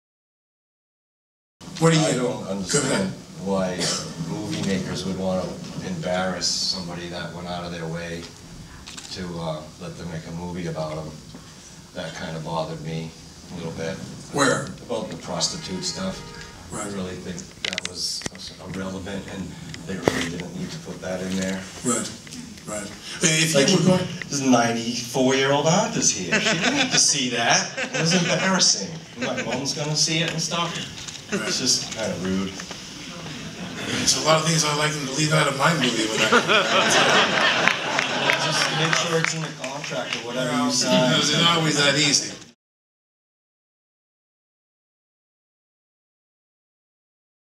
Ready Rebukes Documentarians at Tribeca Film Festival
Jim Ready publicly chides "Compared to What" directors Michael Chandler and Sheila Canavan for dredging up retired Massachusetts Democrat Barney Frank's sordid past.